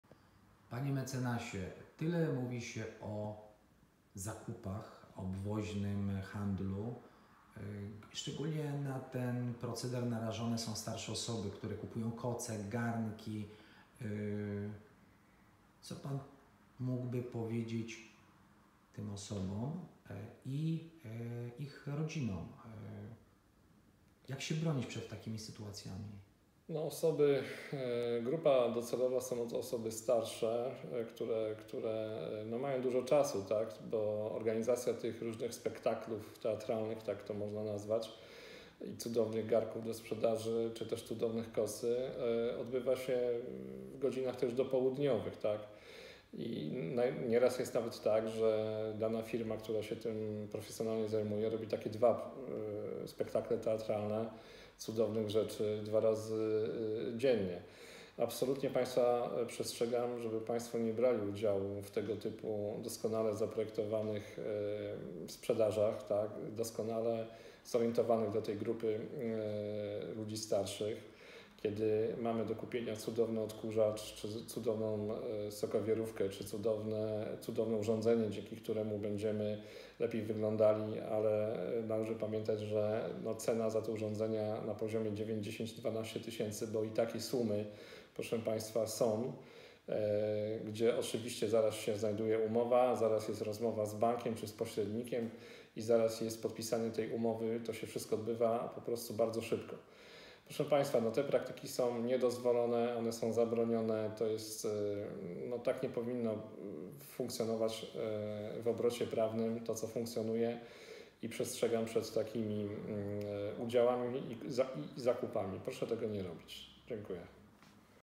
Porady Prawne. Radca prawny Rafał Krupiński. Czy darmowe pokazy dla seniorów do oszustwo? #07
Porady prawne w parafii św. Klemensa Hofbauera w Głogowie. Radca prawny Rafał Krupiński. Czy darmowe pokazy dla seniorów to oszustwo?

katolickiestowarzyszenie